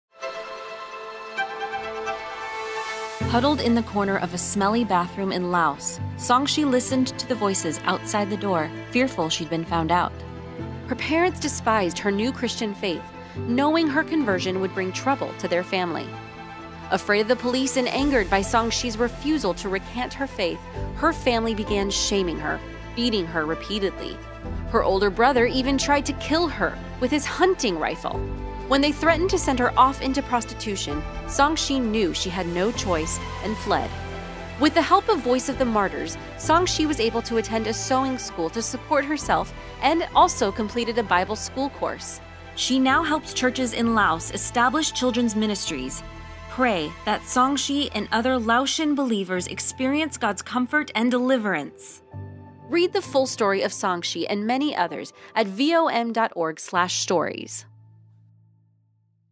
Stories: Laotian Parents Threaten Christian Daughter with Forced Prostitution
Sonxi was forced to hide from her parents in the house church she attended in Laos. Her parents feared the repercussions her conversion to Christianity would have on their family. They threatened to force her into prostitution if she continued to worship God, so Sonxi fled.